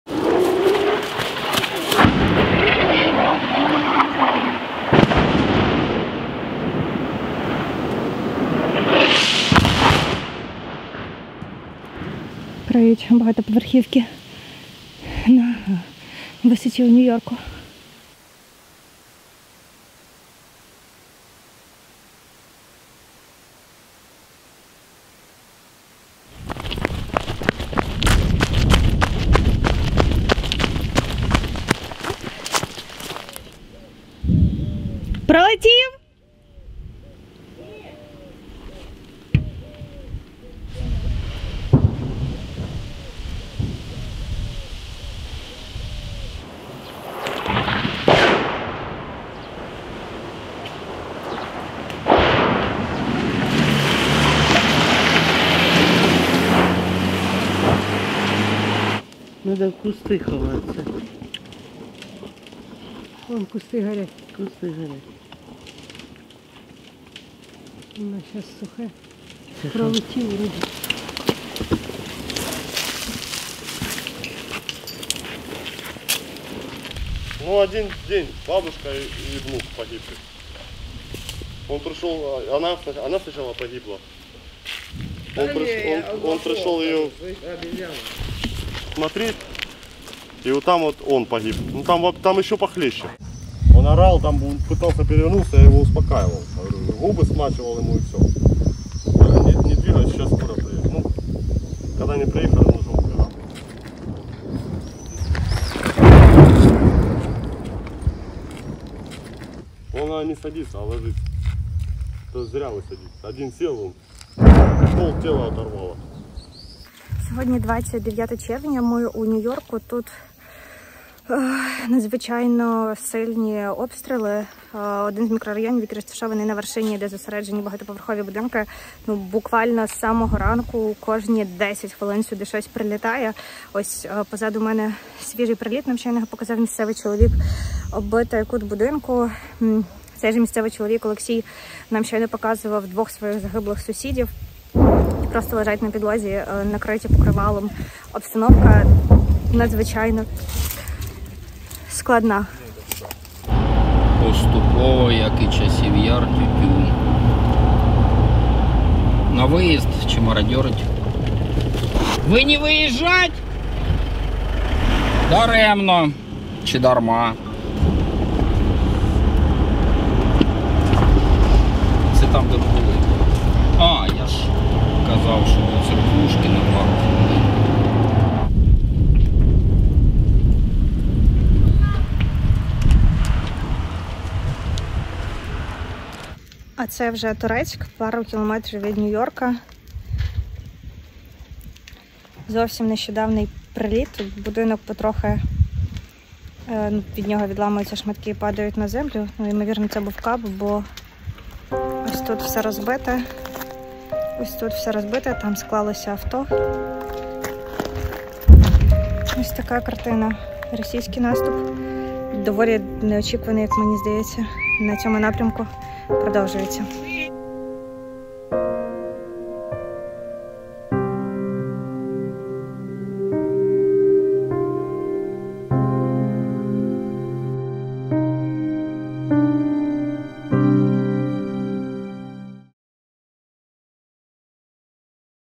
"В один день загинули бабуся і онук" – наступ росіян на Нью-Йорк | УП. Репортаж
Росіяни розпочали наступ на Торецьк та Нью-Йорк, що на Донеччині. Воєнна кореспондентка УП Ольга Кириленко відправилась у прифронтові міста та показала як місцеві мешканці рятуються від снарядів росіян.

Telegram-канал "УП. Off the record"